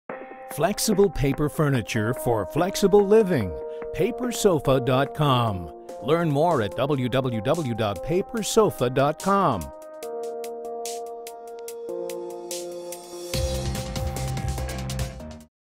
LED Lighted Furniture | Event Furniture Wholesale
Led Lighted modular paper furniture wholesale directly from the manufacturer.
Flexible sustainable eco friendly honeycomb paper furniture. Foldable and expandable.
Free dropship service avialable!
Contact now!
Be Eco, Be Flexible!